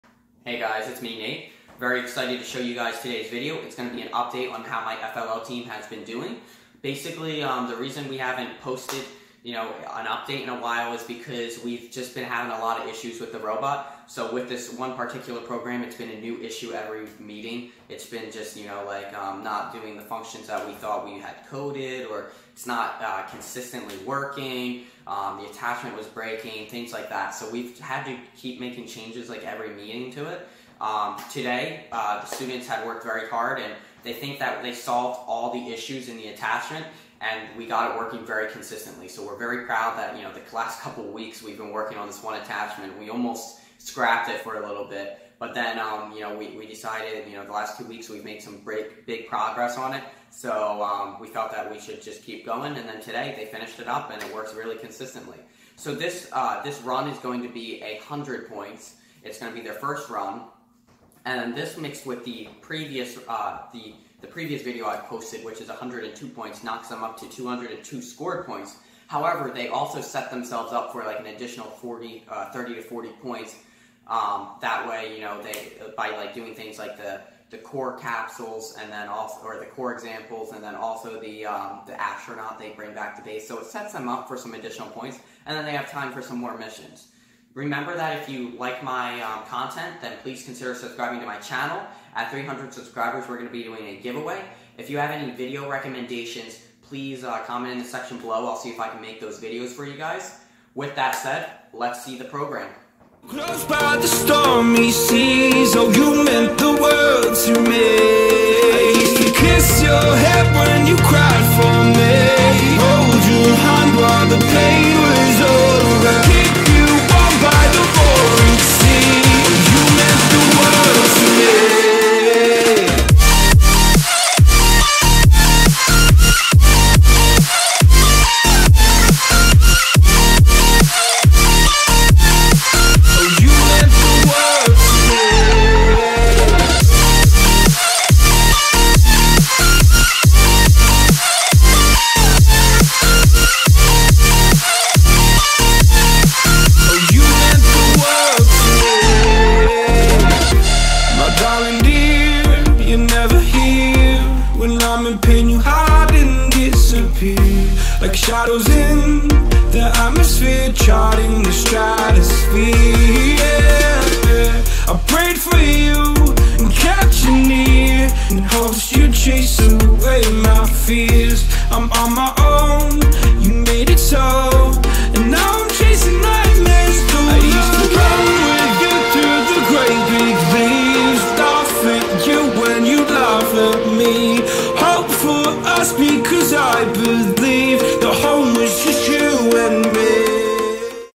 FLL Into Orbit The Road To A High Score: 202 Points In 37 Seconds! [Setup for 234 Points]
This is a update on how my FLL team is progressing throughout the season. We spent many weeks on the big program because we kept having issues. The last 2 weeks the students made alot of progress on the issues we were having and at today's meeting they solved them all and got the run working very consistently. We are now at a total of 202 points scored and set up to score 234 points. Hope you all enjoy and as always if you have questions comment them below.